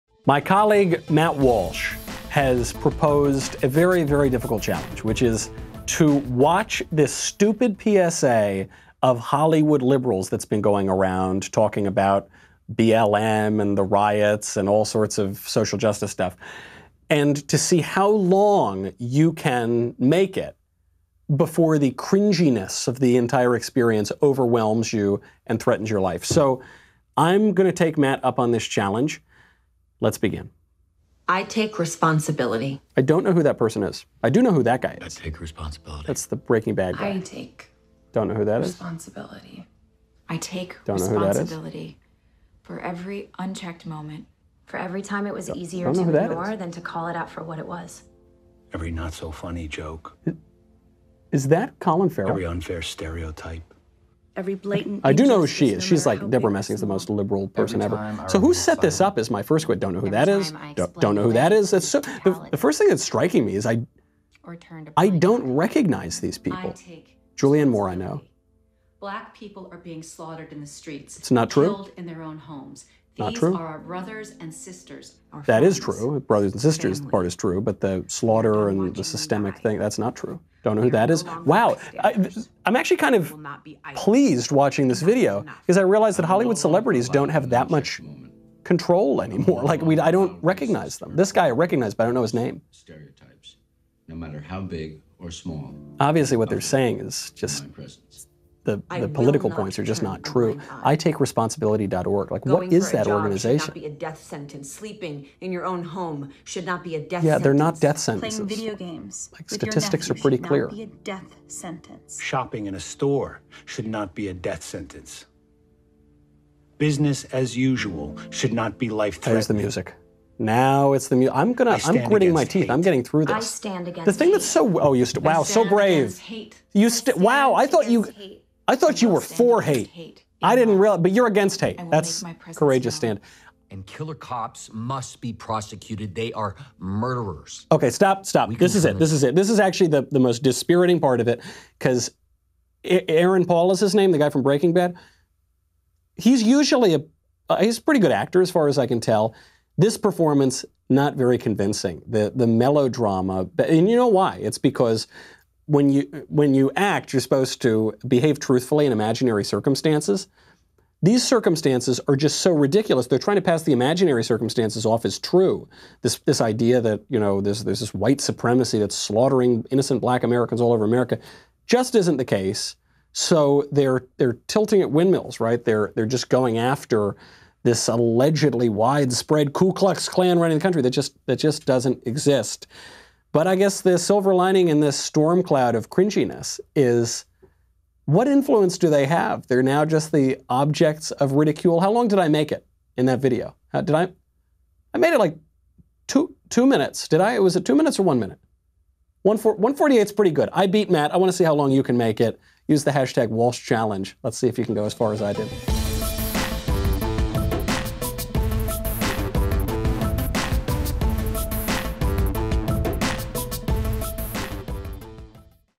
CRINGE: You Can't Watch The Full Video
Matt Walsh posted the most difficult challenge on the internet: to see how long you can watch the celebrity apology cringe video before you are forced to stop it. Post how far you make it and which celebrity finally makes you crack.